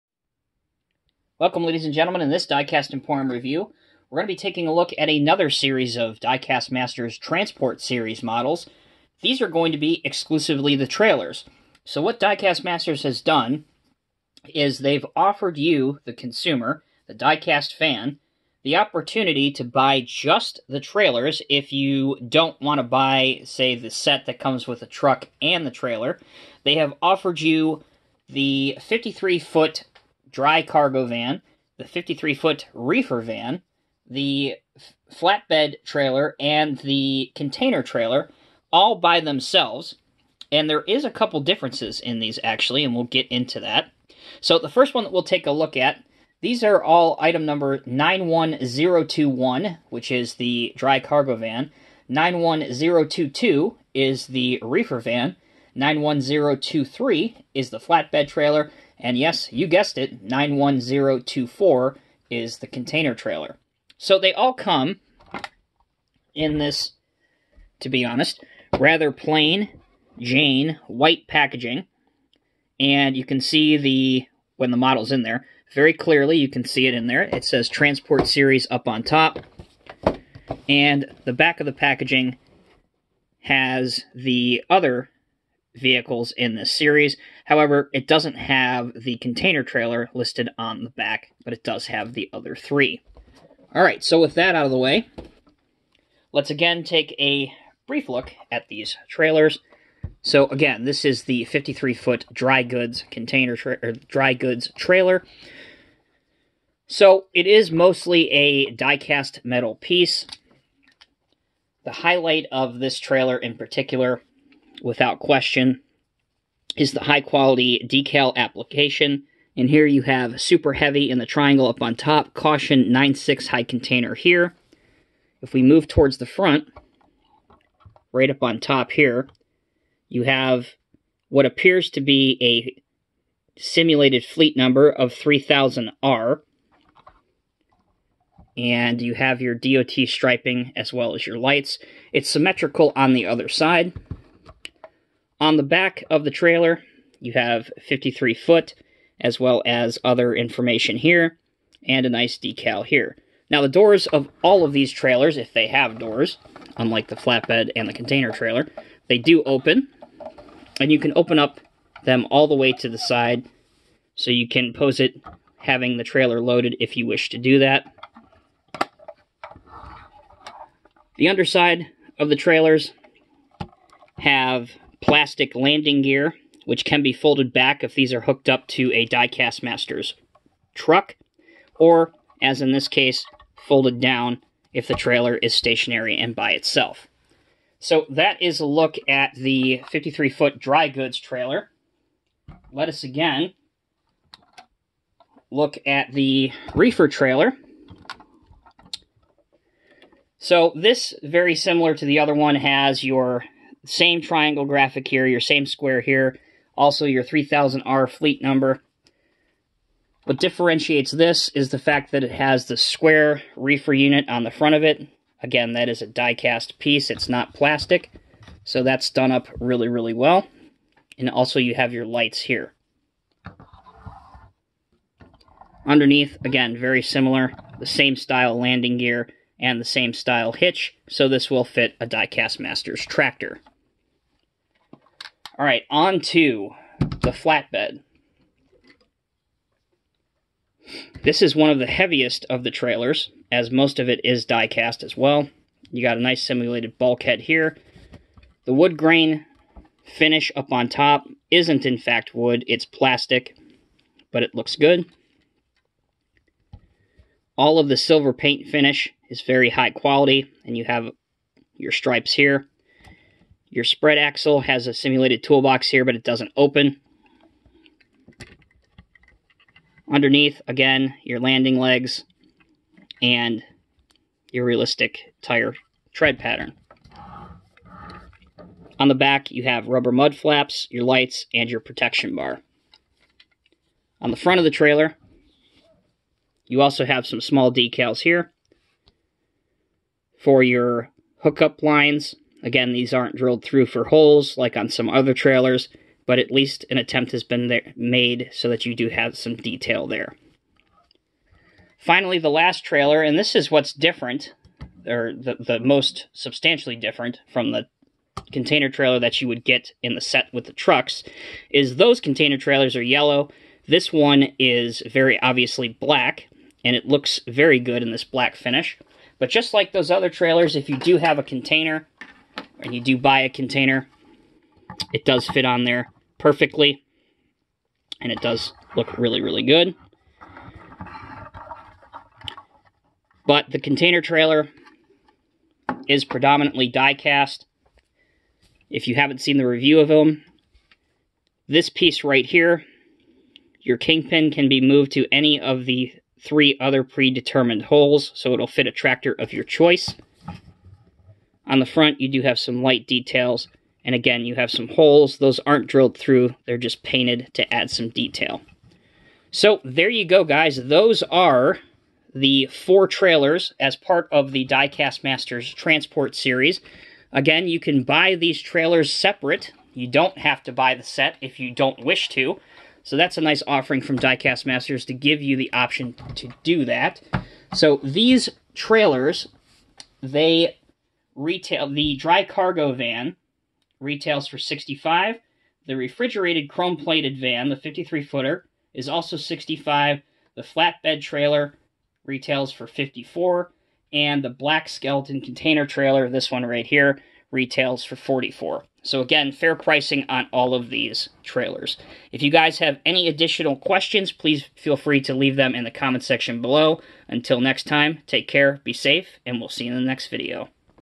Diecast Masters Transport Series Trailer Collection 1:50 Scale
In this review, we look at four trailers offered by Diecast Masters in 1:50 scale. Each of these trailers can be purchased with a tractor, as part of the Transport Series or as is the case here, purchased by themselves. These trailers hook up to any Diecast Masters current truck, including the new International Lonestars and Freightliner Cascadia's. Overall, these trailers are good value for the money, and look great in any collection.

{This video has been brought to you in part by the following:}